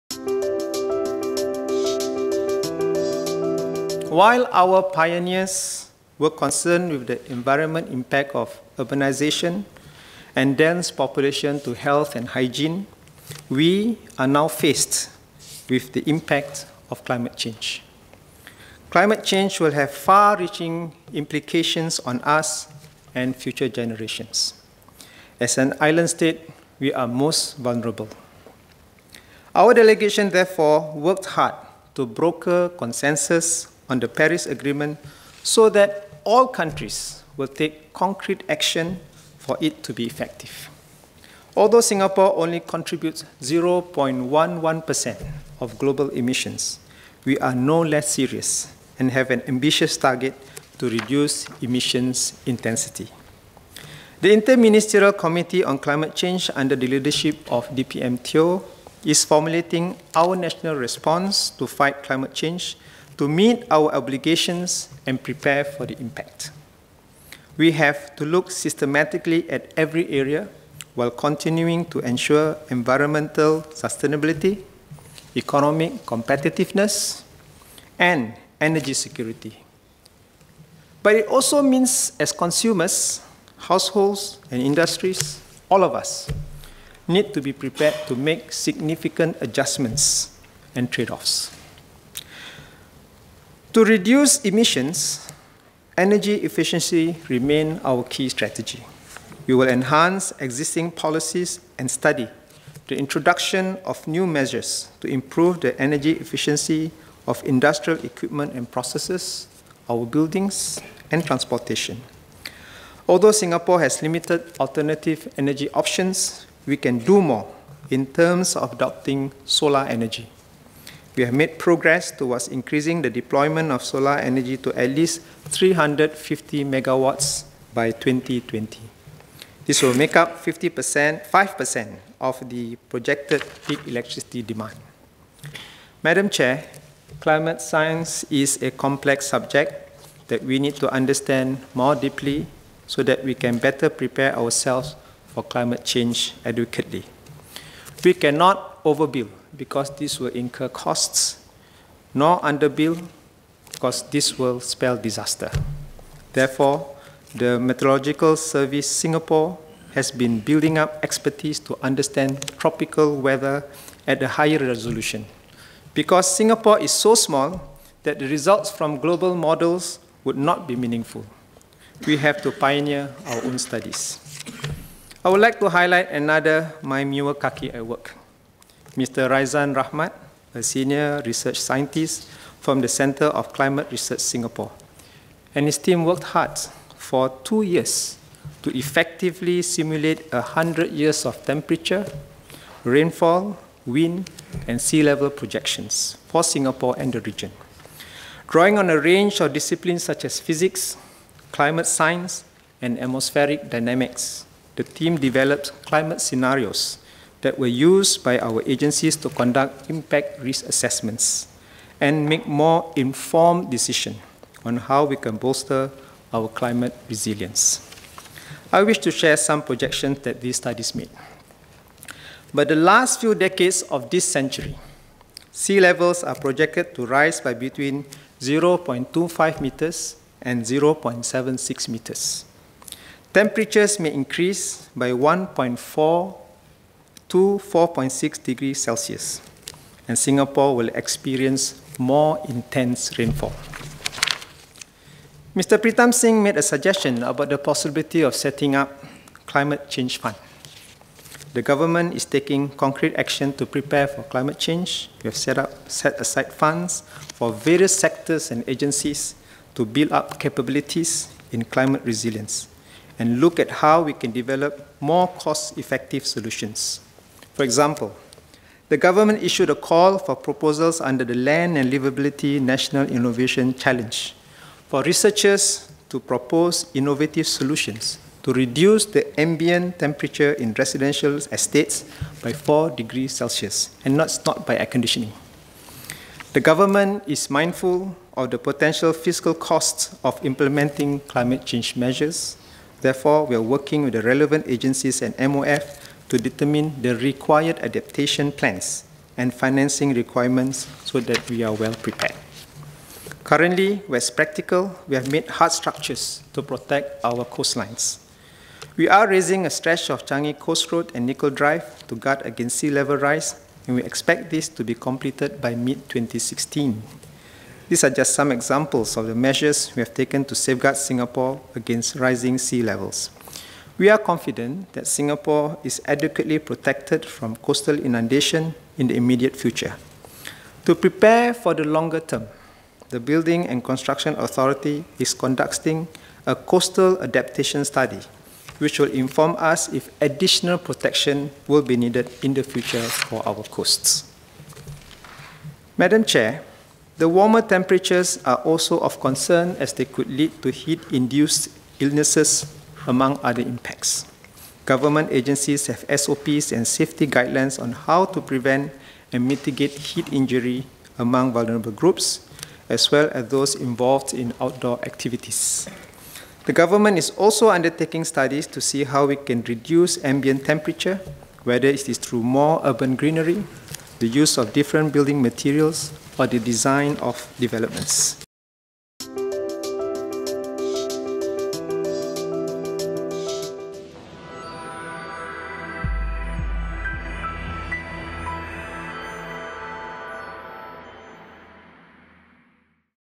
COS 2016: MEWR - Climate Change
Environment and Water Resources Minister Masagos Zulkifli on how the government is responding to the impact of climate change.